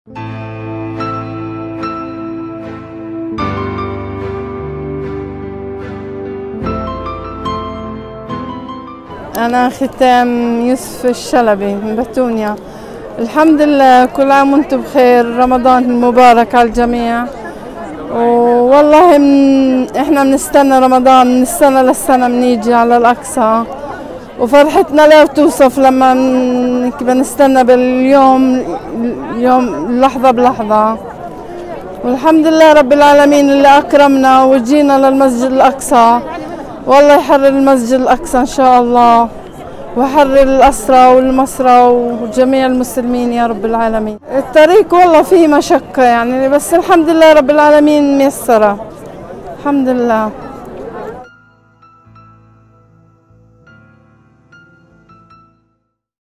ختام الشلبي ننتظر شهر رمضان لنأتي الى الأقصى
ختام الشلبي من بيتونيا قضاء رام اللّه " ننتظر رمضان من السنة للسنة لنيجي عالأقصى.. فرحتنا لا توصف باليوم الذي سنزور الأقصى به.. رغم مشقة الطريق تيسرنا بالوصول للأقصى"